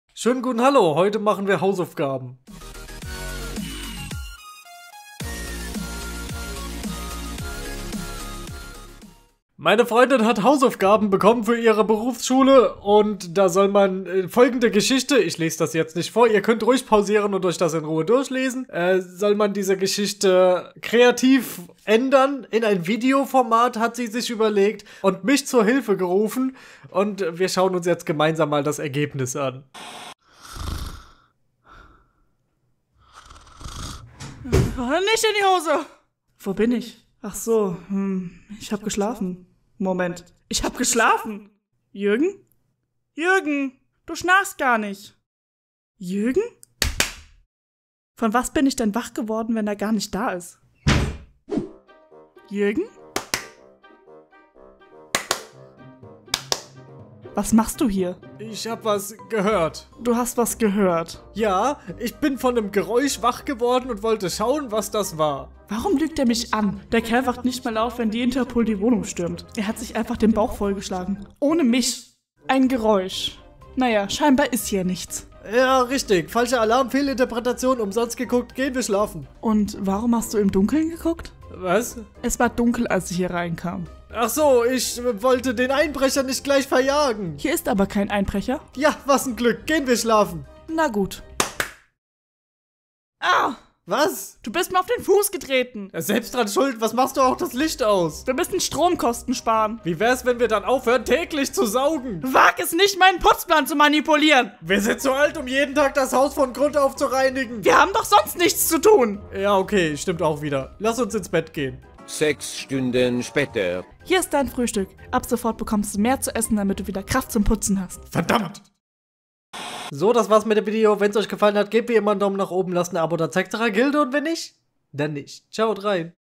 Isabells Hausaufgaben
Meine Freundin Isabell hat Hausaufgaben bekommen für die sie die Kurzgeschichte von Wolfgang Borchert: Das Brot kreativ abändern soll und sie hat sich dafür entschieden mich zur Hilfe zu nehmen und daraus ein kurzes Video zu machen :D

Kurzgeschichte: Wolfgang Borchert - Das Brot